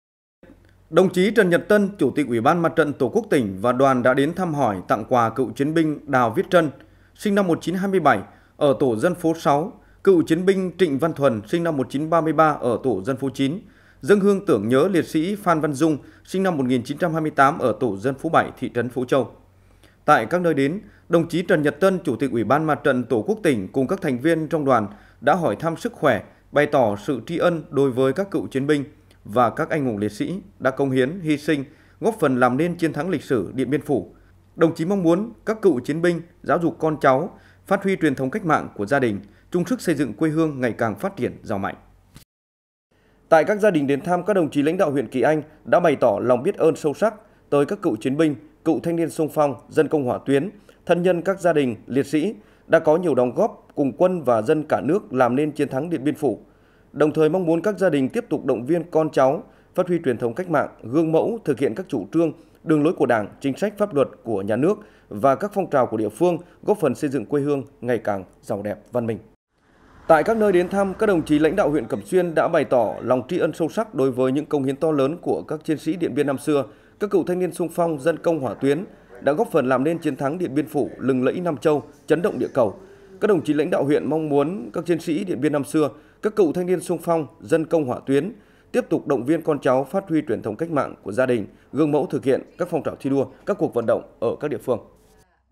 Chủ tịch Ủy ban MTTQ tỉnh Hà Tĩnh tặng quà cựu chiến sĩ Điện Biên | Hà Tĩnh TV
Chủ tịch Ủy ban MTTQ tỉnh Hà Tĩnh tặng quà cựu chiến sĩ Điện Biên
Nhân kỷ niệm 70 năm chiến thắng Điện Biên Phủ, Chủ tịch Ủy ban MTTQ tỉnh Hà Tĩnh Trần Nhật Tân đã đến thăm hỏi, tặng quà và tri ân thân nhân các gia đình liệt sĩ, các cựu chiến binh trên địa bàn thị trấn Phố Châu, huyện Hương Sơn, từng tham gia chiến dịch Điện Biên Phủ.